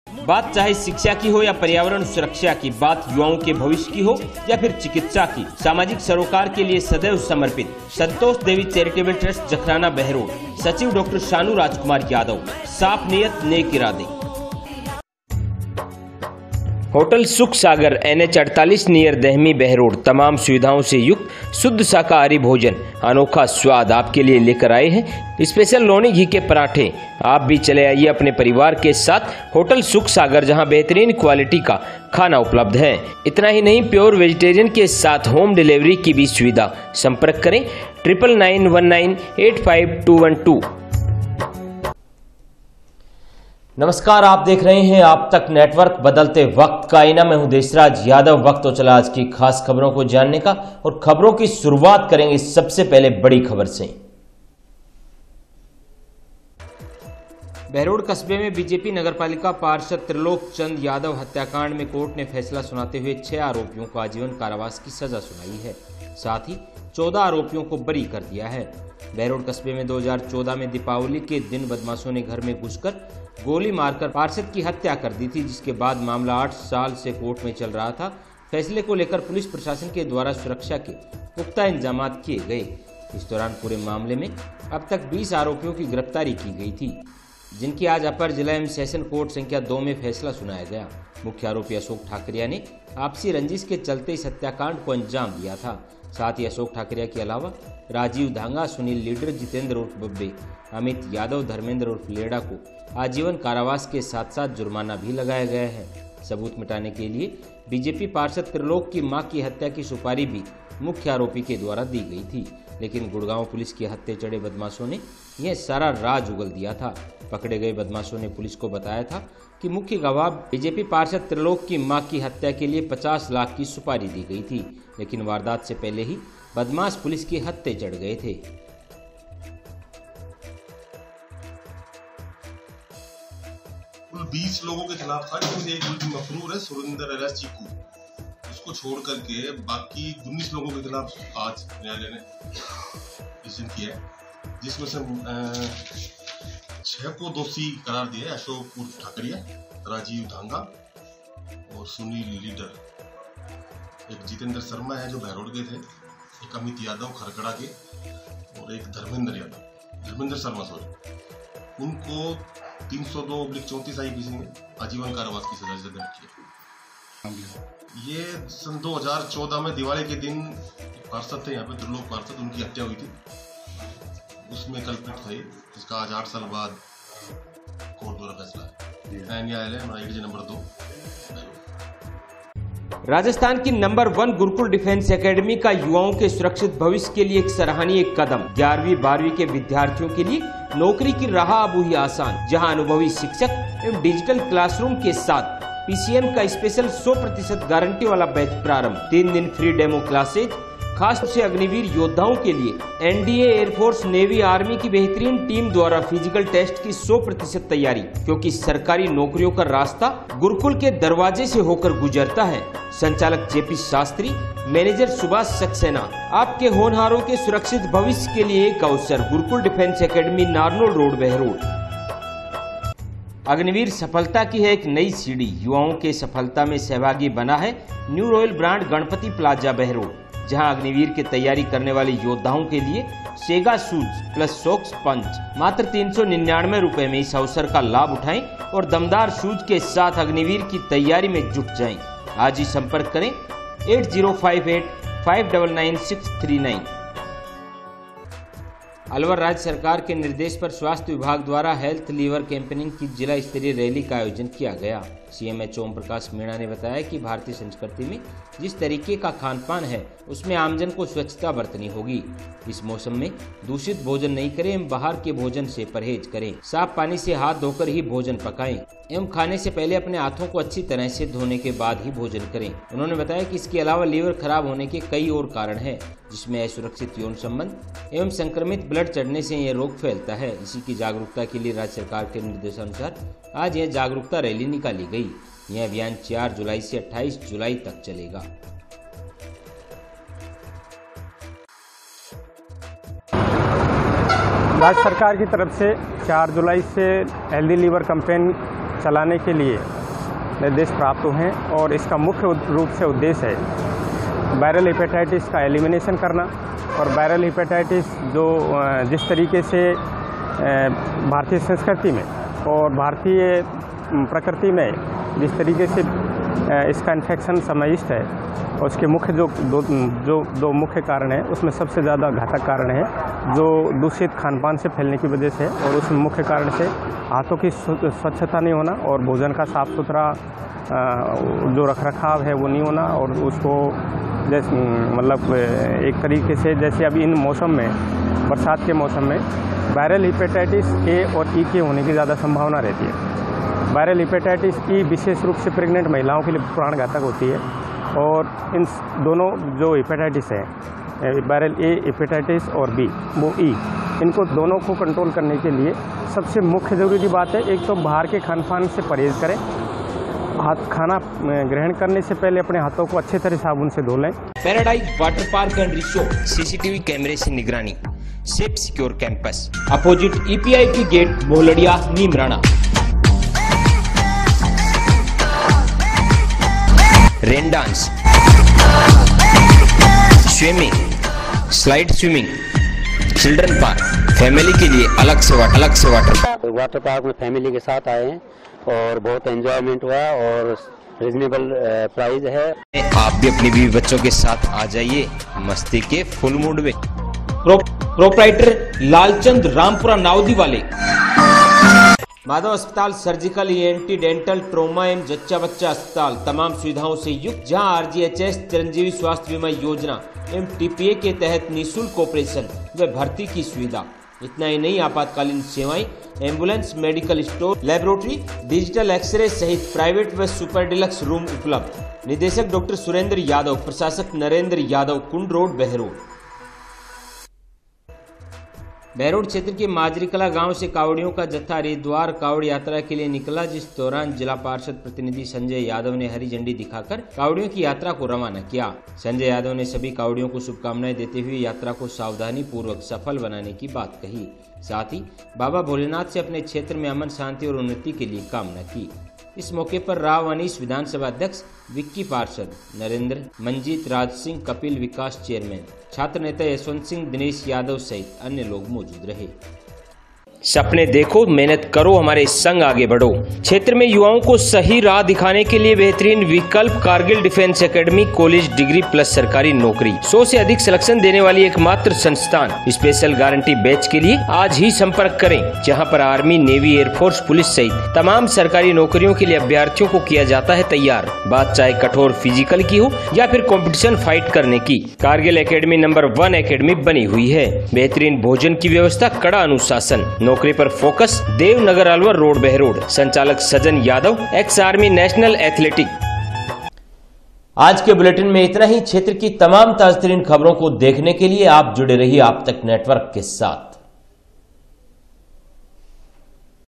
बहरोड़ बीजेपी पार्षद की हत्याकांड में आरोपी अशोक ठकरिया सहित अन्य आरोपियों को सुनाई सजा,बड़ी खबर
अशोक ठकरिया सहित अन्य आरोपियों को आजीवन कारावास की सजा सुनाई